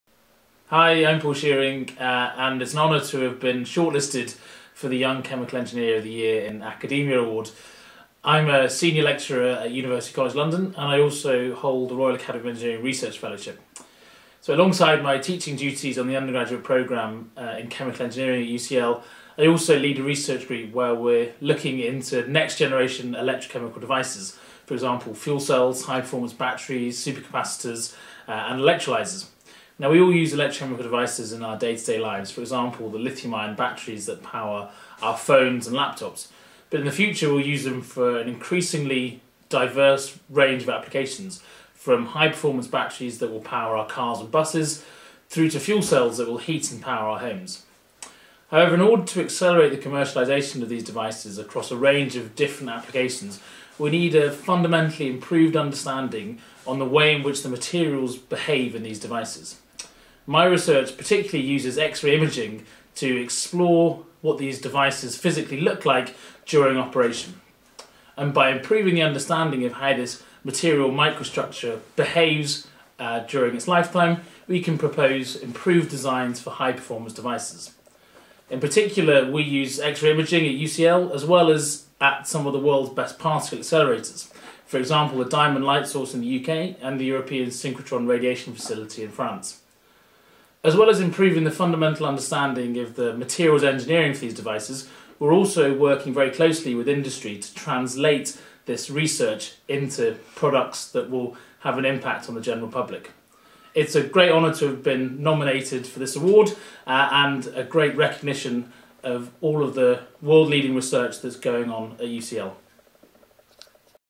IChemE Global Awards 2014 - Paul Shearing
Paul Shearing, senior lecturer in chemical engineering - University College London

Paul has pioneered the application of multi-length scale microscopy to explore materials for fuel cells and batteries. The devices have been identified by the UK Government as core technologies for the reduction of CO2 emissions.

Paul’s work has attracted significant interest from industry, including General Motors, Tata Steel and Praxair and has received extensive research funding from EPSRC, MRC, RAEng, Office of Naval Research and STFC.

Paul is a recognised international expert in his field and has published more than 50 peer reviewed papers and is the recipient of a 2012 US DoE Stanford Science Highlight Award.

In 2014 he was nominated for UCL’s Student Choice Teaching awards Roll of Honour.